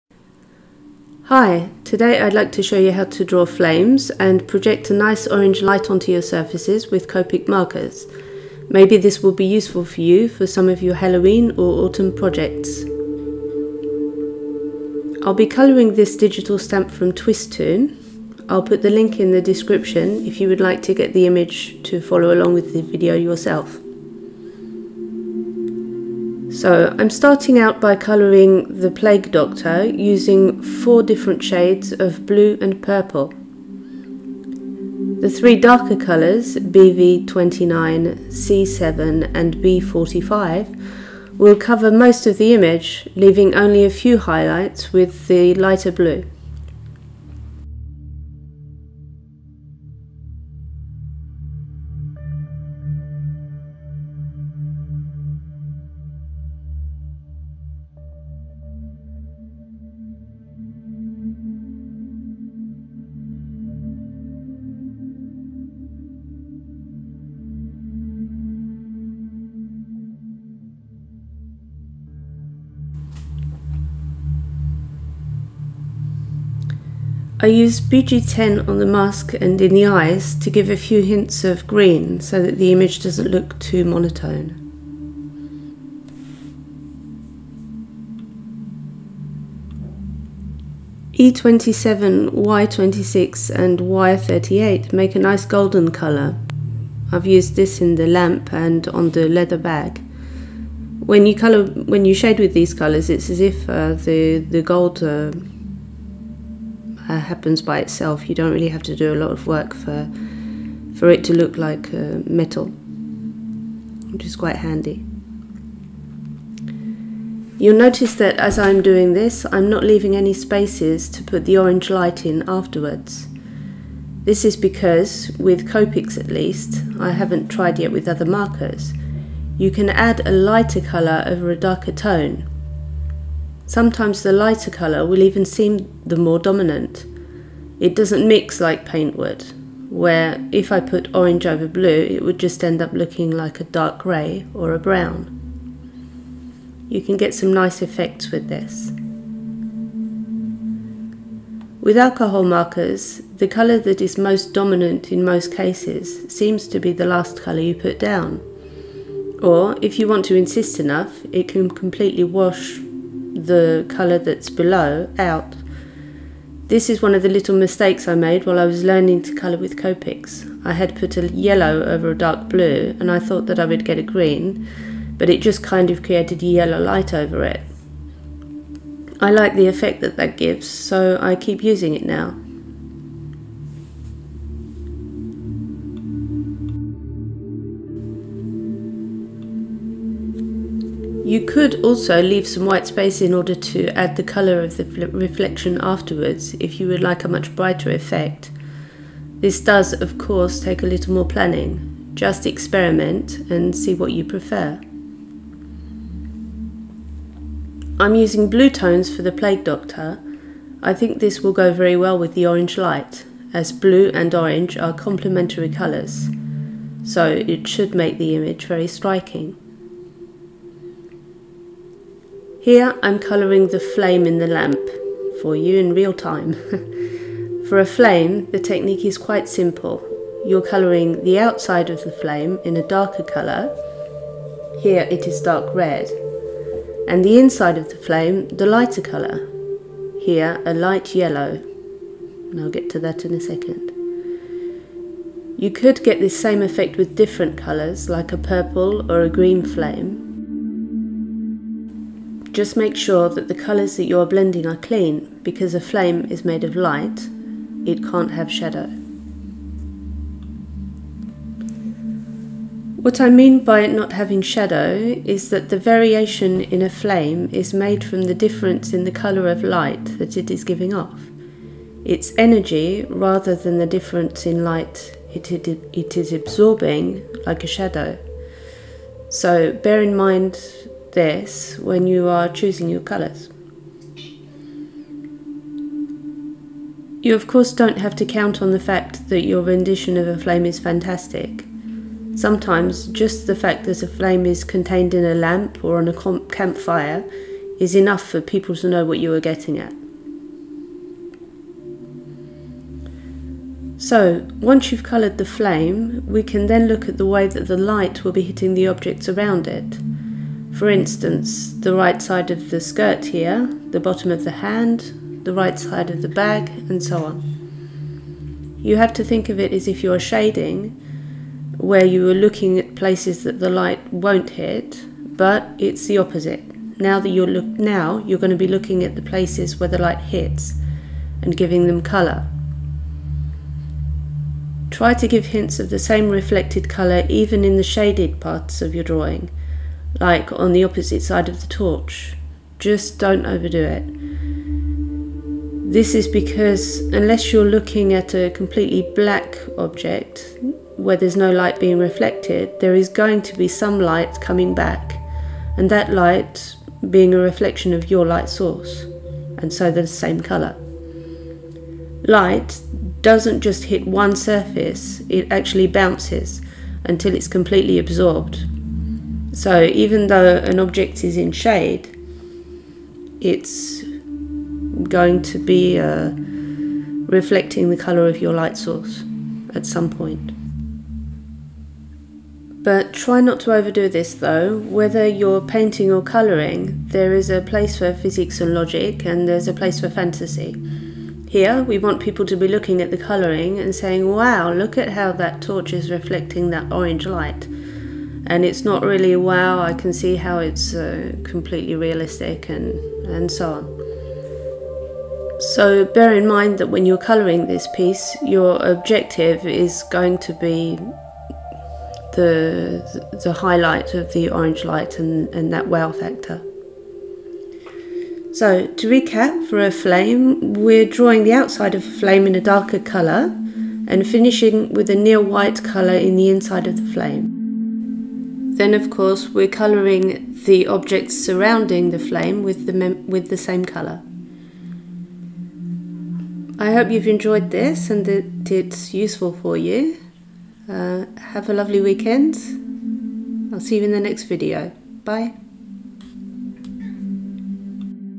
COPIC TUTORIAL - HOW TO COLOR TORCHLIGHT AND FLAMES
How to color colored torch light and flames with copic markers.